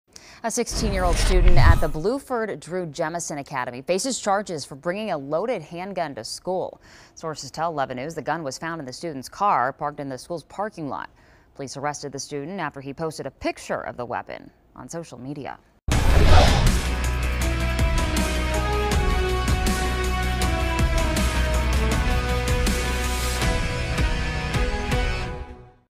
Police: Teen arrested with loaded semiautomatic handgun at school
Baltimore City police arrested a 16-year-old student who had a loaded semiautomatic handgun at a school Thursday morning. Sources told 11 News the gun was found in the student's car, which was parked in Bluford Drew Jemison STEM Academy's parking lot in the 1500 block of Harlem Avenue. Sources also told 11 News that police were able to make the arrest after the student posted a photo of the weapon on social media.